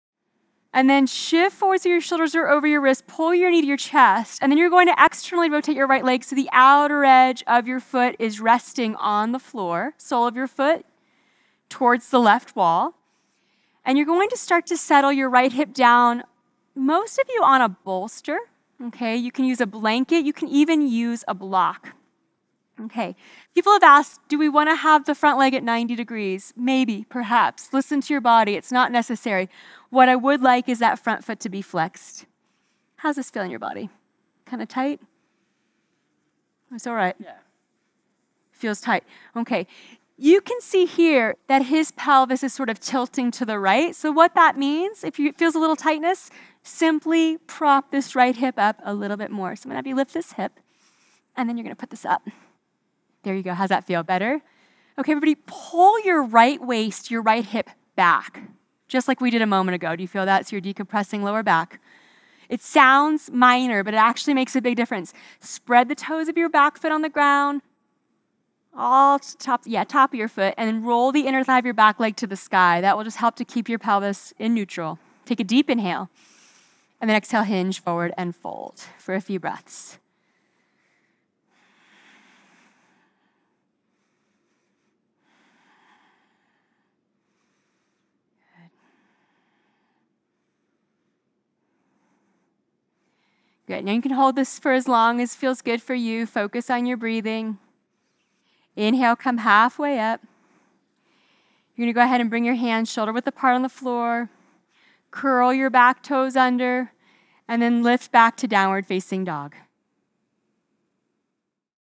Gigi Snyder, How To Be a Hipster I UDAYA.com
Open your hips and have a little fun with this deep stretch for hips and hamstrings with Gigi Snyder.